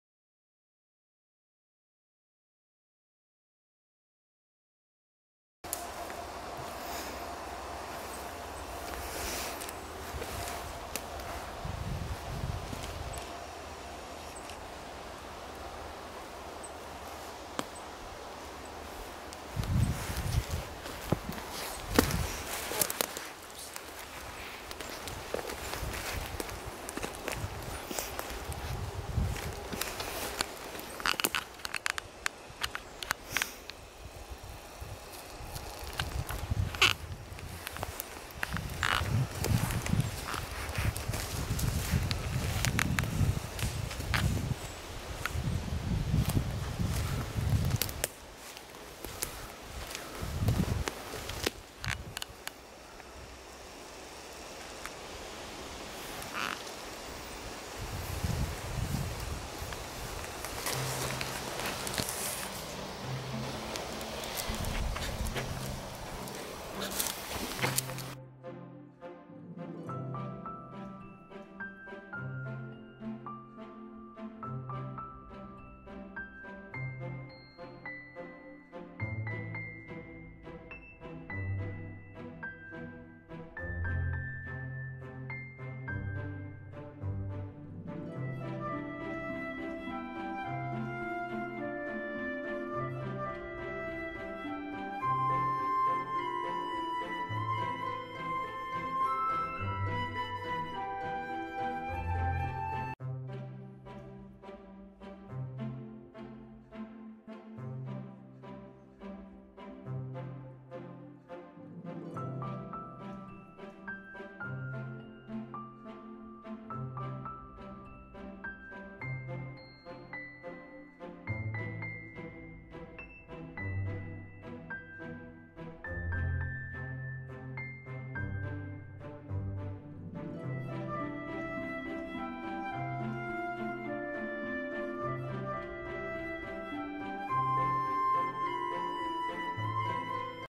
MUSHROOM HUNTING I WHAT MUSHROOMS CAN YOU FIND IN THE FOREST
Mushroom picking, forest in Poland. Eatable mushroom hunting.